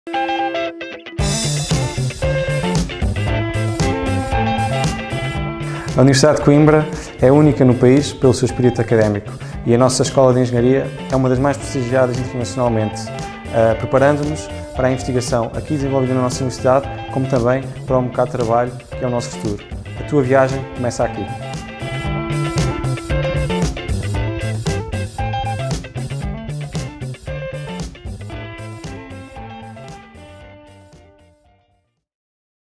13.º - A Tua Viagem começa na Engenharia Eletrotécnica e de Computadores
A escola de engenharia da Universidade de Coimbra é única. As palavras são do João Martins, do Núcleo de Estudantes de Engenharia Electrotécnica e de... da FCTUC.

De 17 de julho a 6 de agosto decorre a 1.ª fase de candidaturas ao Ensino Superior. A tua viagem começa aqui... na Universidade de Coimbra. Atreves-te a dar o primeiro passo?